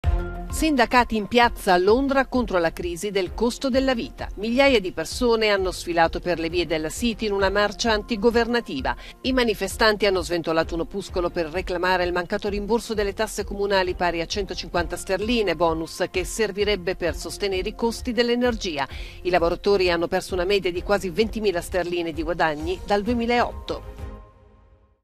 Londra, la protesta dei sindacati contro il caro-vita
Proteste contro il caro-prezzi a Londra. I sindacati puntano il dito contro i conservatori: "i Tory paghino questa crisi"